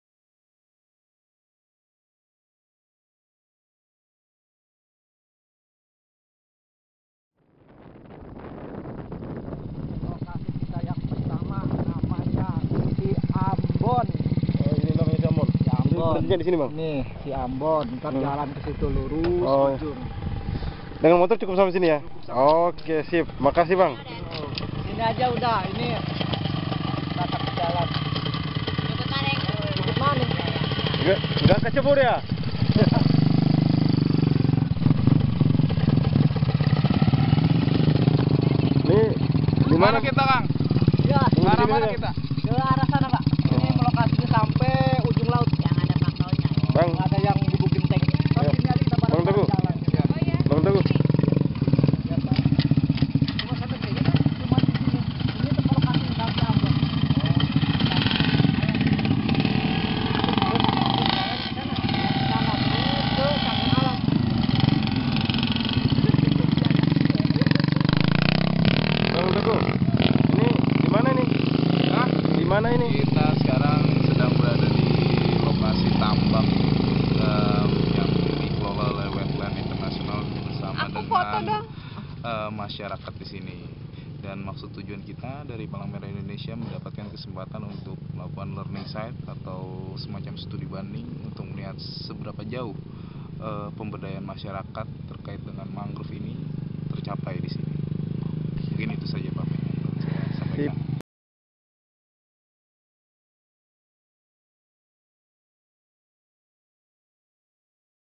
Learning Site Partner for Resilience
Aliansi Kemitraan untuk Ketahanan melakukan kunjungan lapangan terkait dengan pemberdayaan masyarakat untuk tahan terhadap bencana. Kelompok masyarakat binaan Wetlands Indonesia.